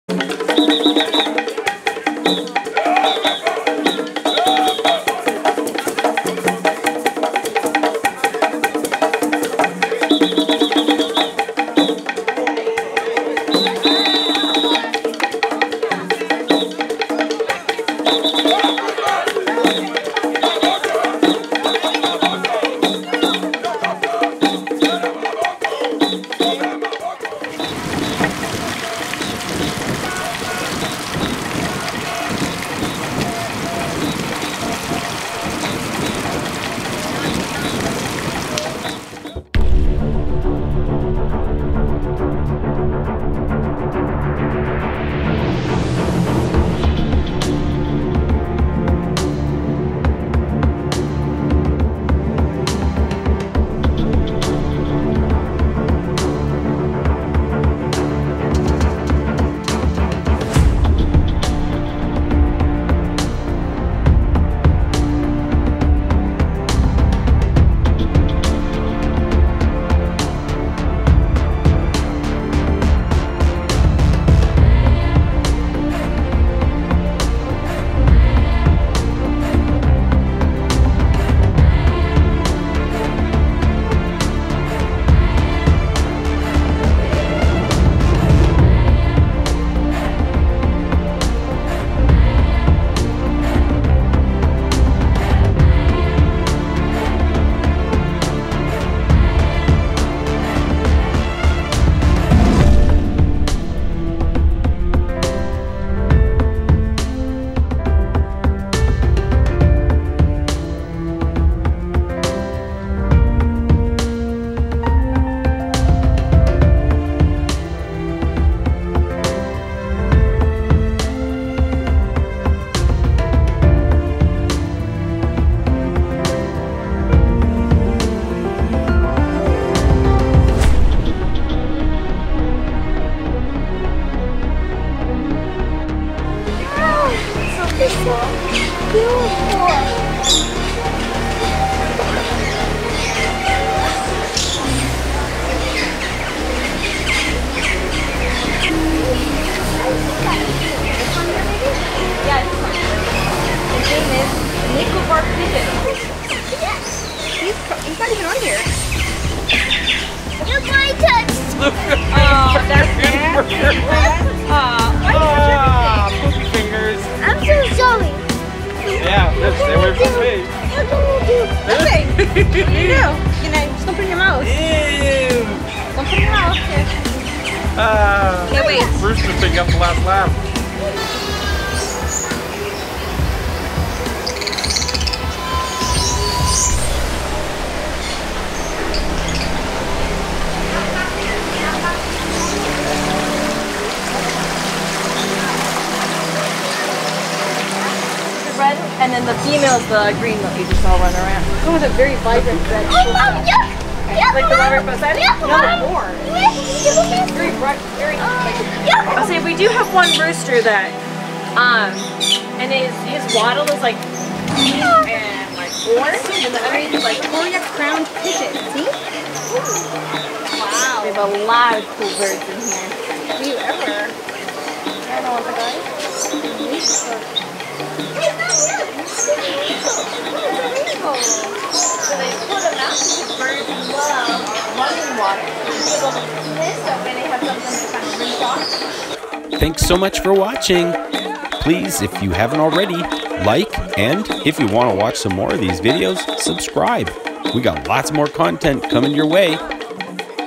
Maharajah JUNGLE Trek at DISNEY'S ANIMAL KINGDOM 2019 in 4k
Join us for a walking Tour of the Maharajah Trek at Disney's Animal Kingdom Park.  This is a great walking adventure that should not be missed.  It takes about 20 minutes to complete but can be longer if depending on how much time you wish to look at the various animals you will see along the trail.